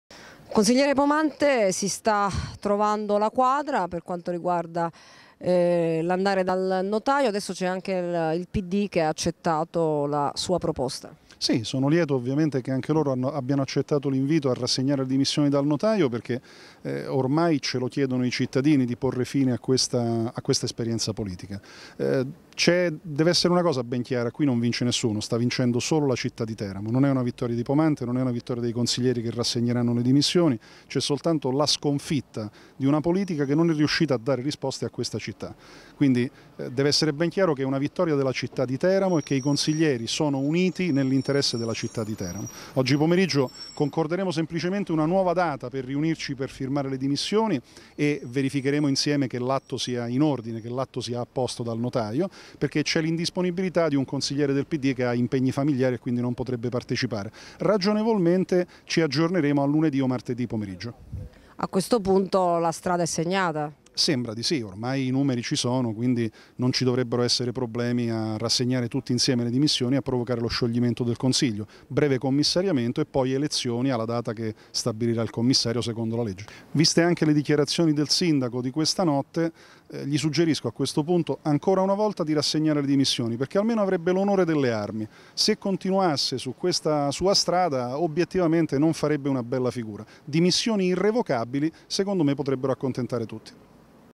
SFIDUCIA A BRUCCHI, IL PD ACCOGLIE L'INVITO DI GIANLUCA POMANTE
LA DATA DEL NUOVO APPUNTAMENTO SARÀ FISSATA NEL POMERIGGIO

Servizio di J News - 22 novembre 2017

| SUPER J |

Contatti Redazione:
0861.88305

Seguiteci su